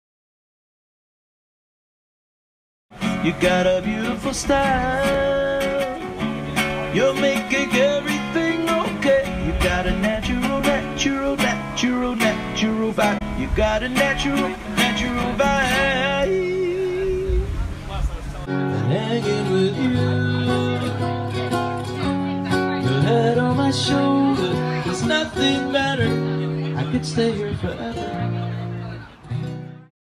BRUNCH MUSIC • Live Acoustic Soulful Beach Groove
Elevate your brunch experience to a joyful celebration with Soulful Beach Groove music from Evyn Charles!